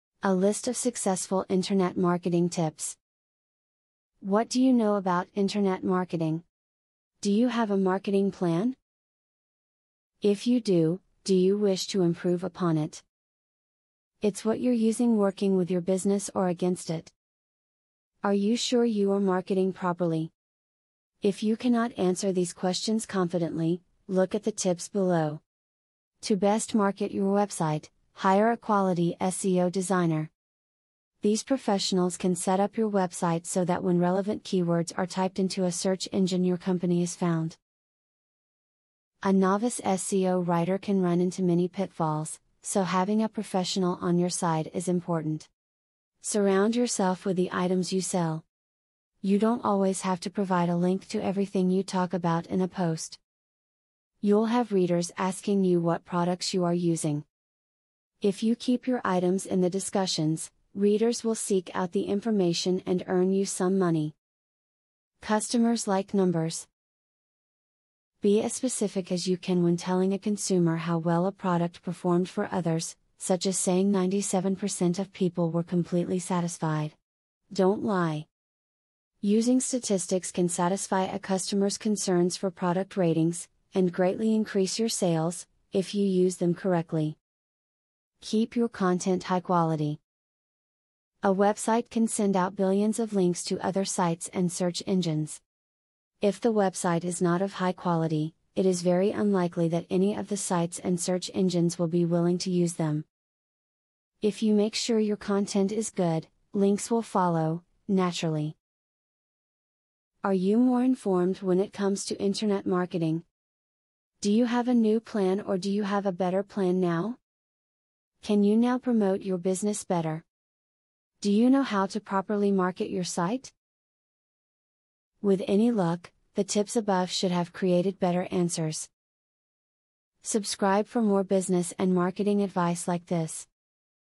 A List Of Successful Internet Marketing Tips.
A List Of Successful Internet Marketing Tips. What do you know about internet marketing? Do you have a marketing plan? If you do, do you wish to improve upon it? It's what you're using working with your business or against it?